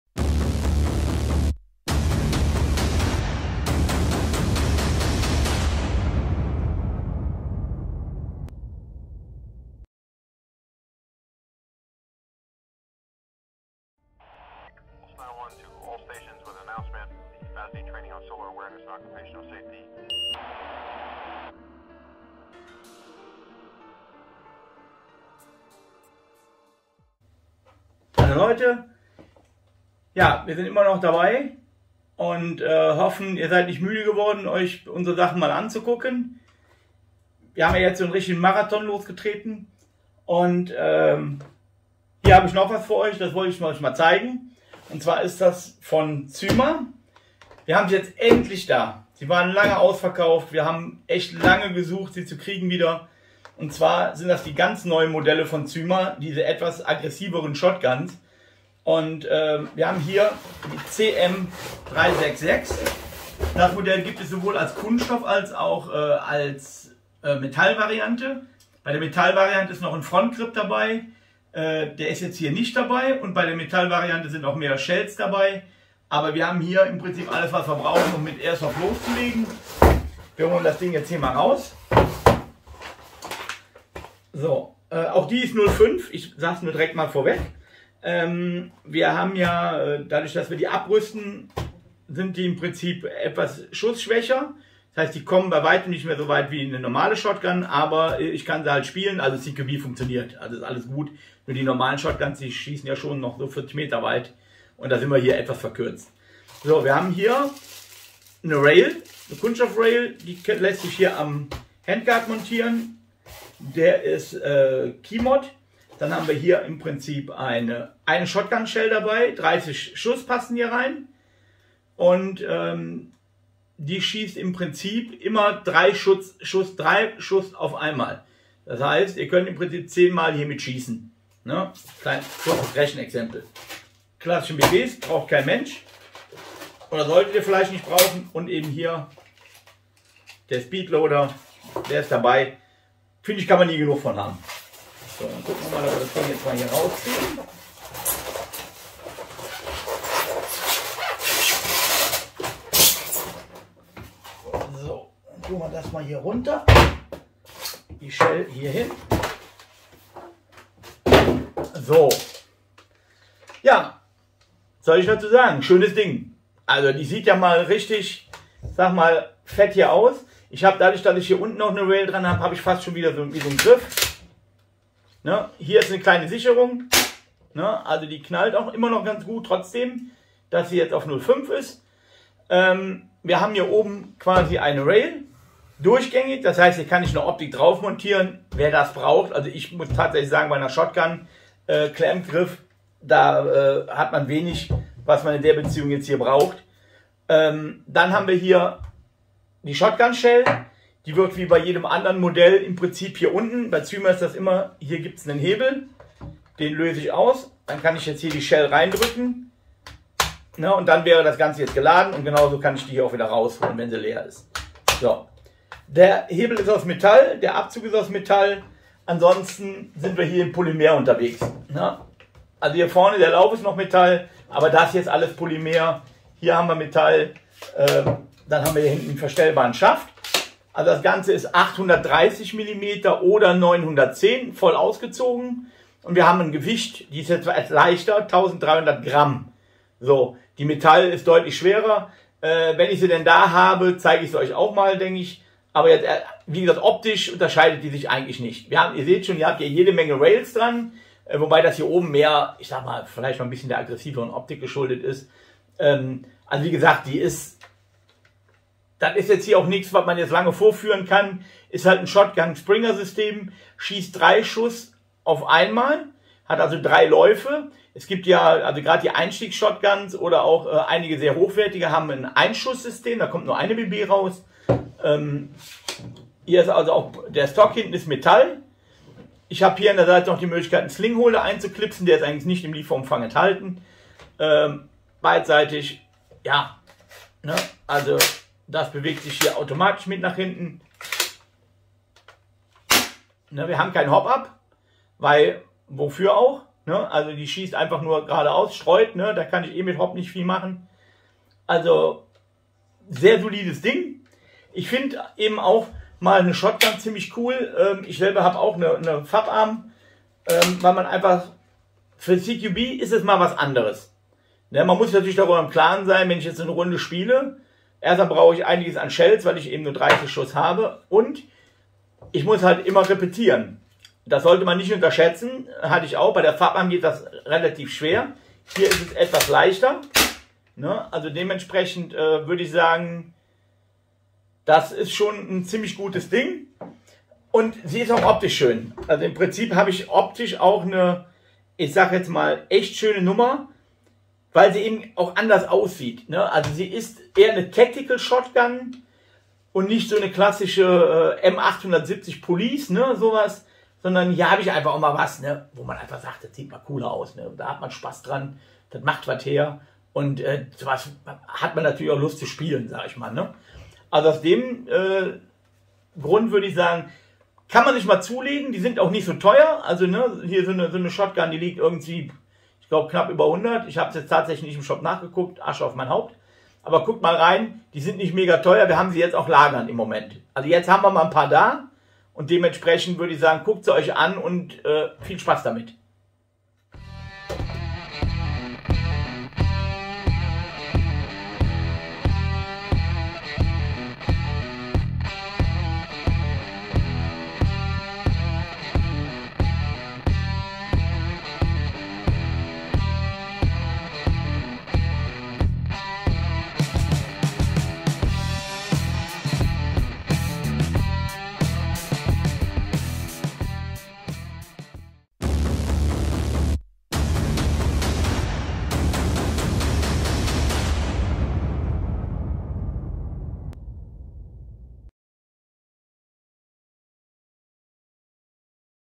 Cyma CM366 Tactical Shotgun Polymer Airsoft Softair ab 14 Review Test Softair.zone Deutsch German
Wir stellen euch hier kurz die neue Generation von Shotguns der Firma Cyma vor. Dieses Modell gibt es als Polymer (stellen wir hier vor ) und Metall Version.
Besonders and dieser Shotgun ist der moderne aggressive Handguard.
Wir hoffen das euch das Video gefällt.